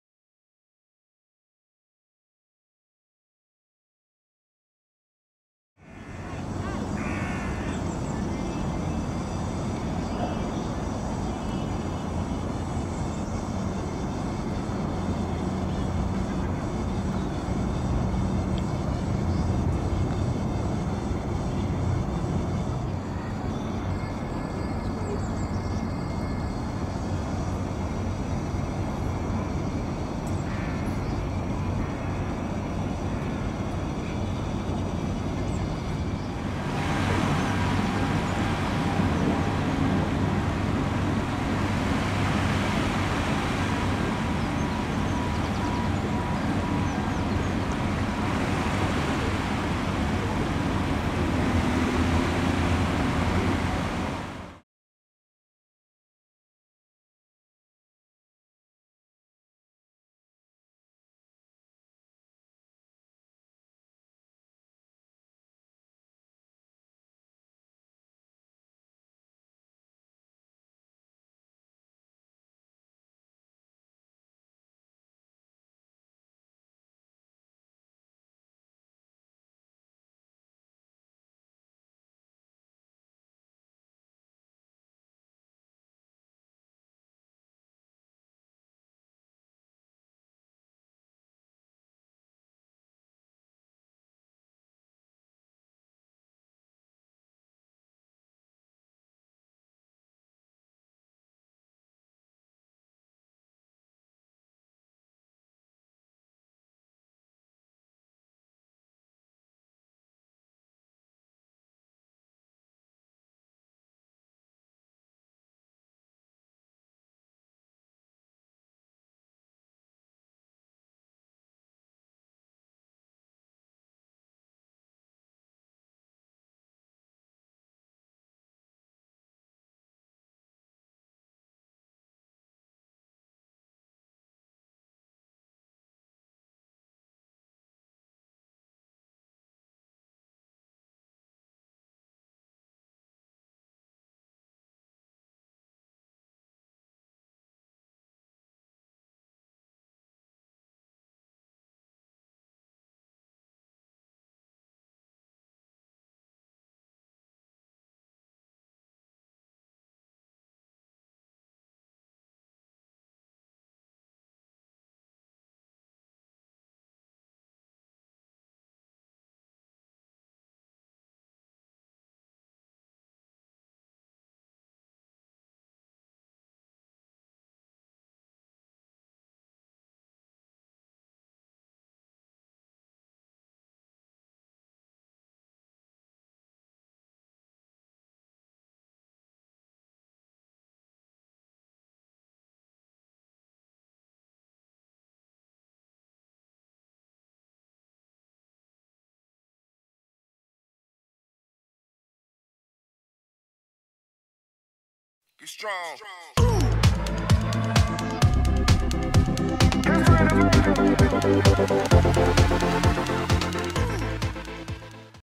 North Sea Atlantic - Offshore Support Vessel inbound to Liverpool docks
NORTH SEA ATLANTIC (IMO: 9665073) is a Offshore Support Vessel registered and sailing under the flag of  Norway. Her gross tonnage is 15701 and deadweight is 9300. NORTH SEA ATLANTIC was built in 2014. NORTH SEA ATLANTIC length overall (LOA) is 143.75 m, beam is 27.04 m. Her container capacity is 0 TEU. The ship is operated by NORTH SEA SHIPPING AS.